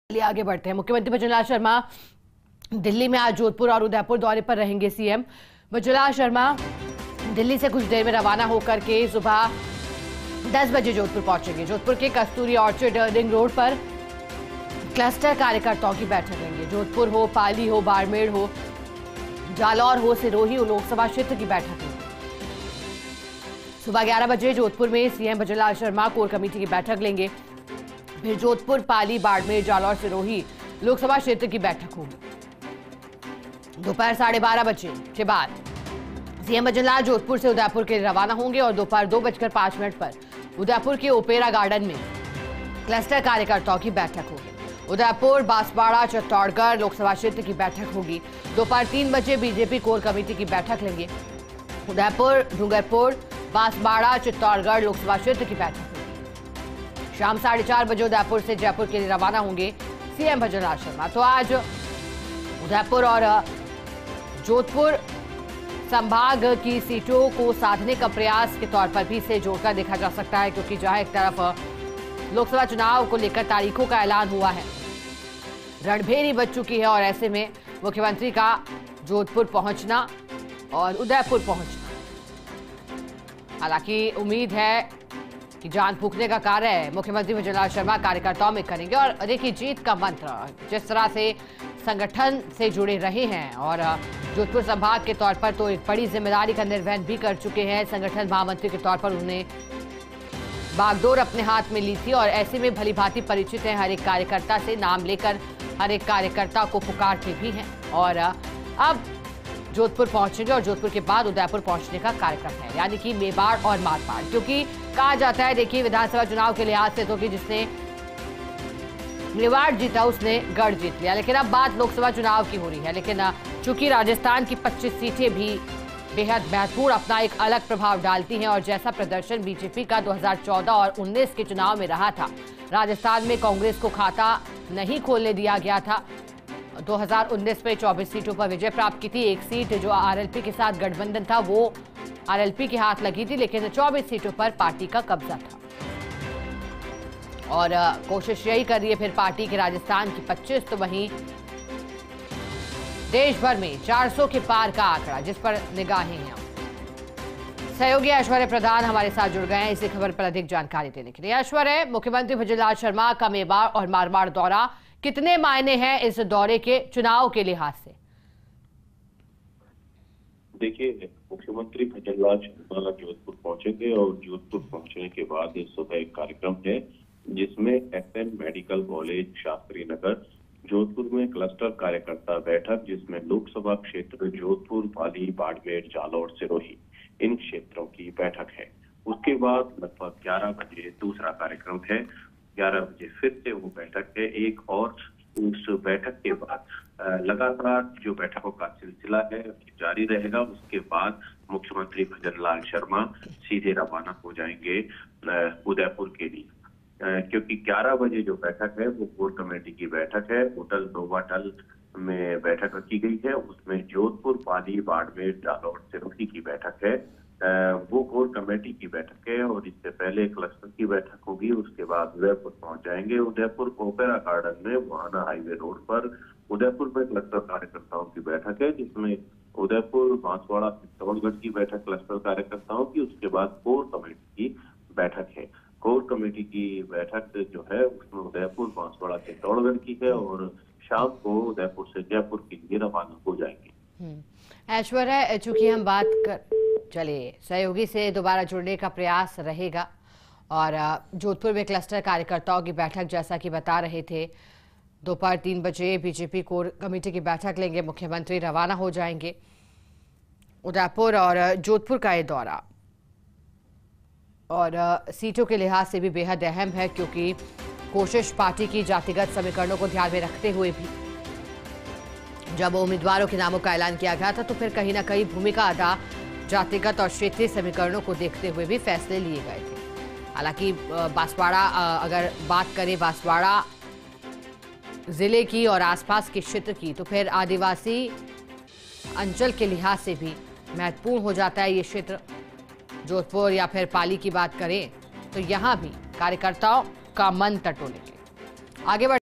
आज CM BhajanLal Sharma रहेंगे Jodhpur और Udaipur दौरे पर | Loksabha Election 2024
About this Video:

मुख्यमंत्री भजनलाल शर्मा दिल्ली में,आज जोधपुर और उदयपुर दौरे पर रहेंगे सीएम भजनलाल शर्मा,दिल्ली से कुछ देर में रवाना होकर सुबह 10 बजे पहुंचेंगे जोधपुर ,जोधपुर के कस्तूरी-ऑर्चिड रिंग रोड पर क्लस्टर कार्यकर्ताओं की लेंगे बैठक ,जोधपुर, पाली, बाड़मेर, जालोर-सिरोही लोकसभा क्षेत्र की लेंगे बैठक ,सुबह 11 बजे जोधपुर में सीएम भजनलाल शर्मा लेंगे कोर कमेटी की बैठक,जोधपुर, पाली, बाड़मेर, जालोर-सिरोही ,कसभा क्षेत्र की होगी बैठक ,दोपहर 12:50 बजे सीएम भजनलाल जोधपुर से उदयपुर के लिए होंगे रवाना, दोपहर 2:05 बजे उदयपुर के ओपेरा गार्डन में क्लस्टर कार्यकर्ताओं की बैठक, उदयपुर, बांसवाड़ा, चित्तौड़गढ़ लोकसभा क्षेत्र की होगी बैठक ,दोपहर 3 बजे बीजेपी कोर कमेटी की लेंगे बैठक,उदयपुर, डूंगरपुर-बांसवाड़ा, चित्तौड़गढ़ लोकसभा क्षेत्र की बैठक
शाम 4:30 बजे उदयपुर से जयपुर के लिए रवाना होंगे सीएम भजनलाल शर्मा